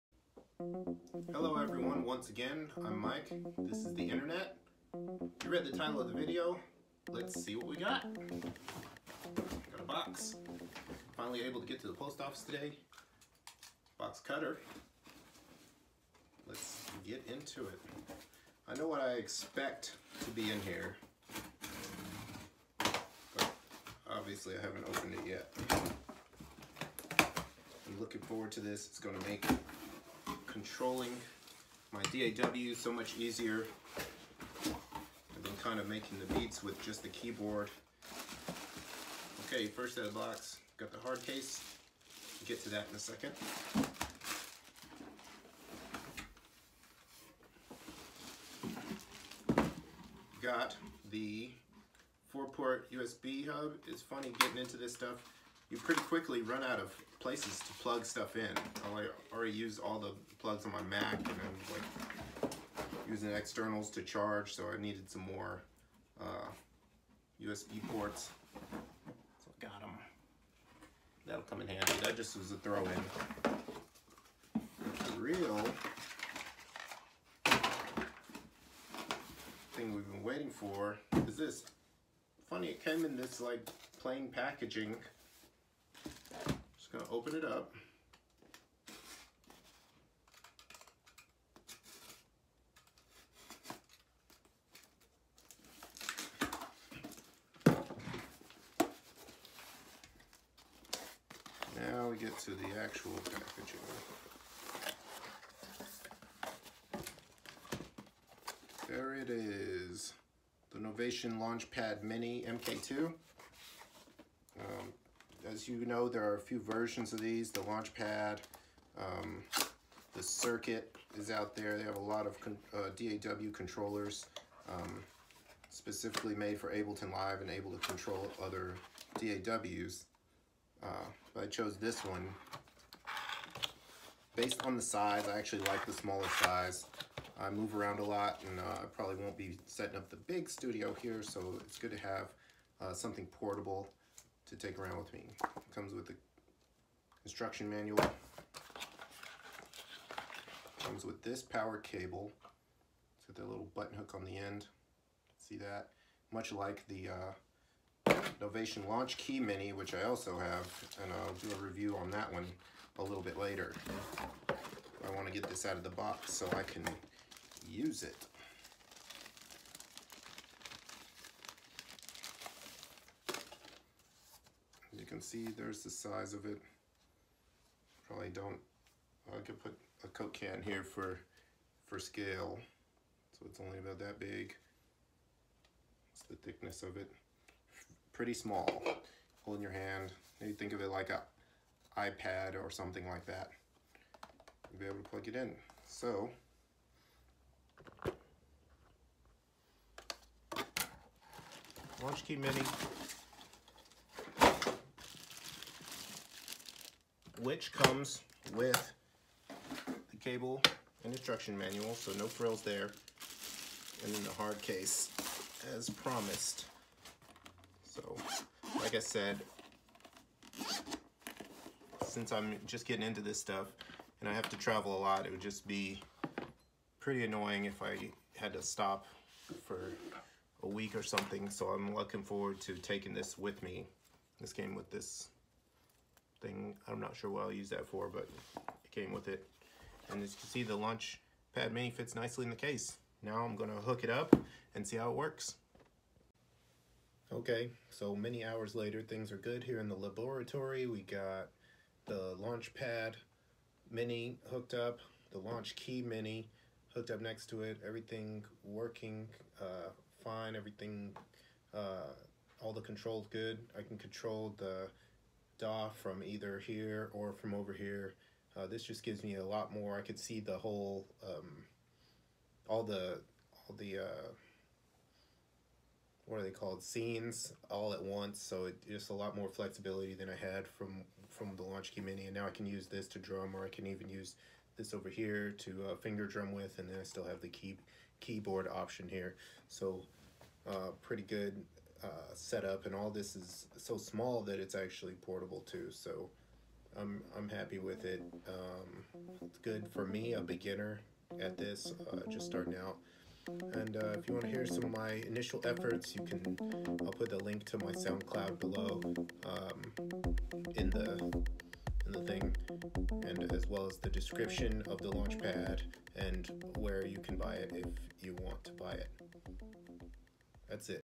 UNBOXING the Novation Launchpad Mini MKII
UNBOXING the Novation Launchpad Mini MKII DAW Controller and whatever else I happened to buy at the same time.